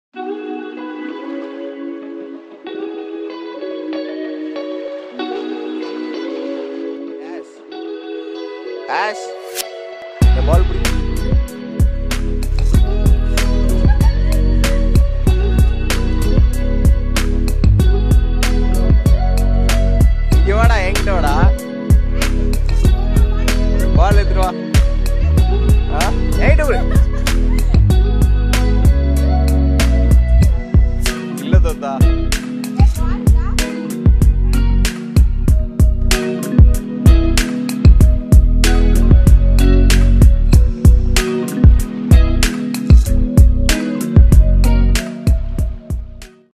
playfull dog
Ash having a good time with his siblings and showing his playful side to everyone.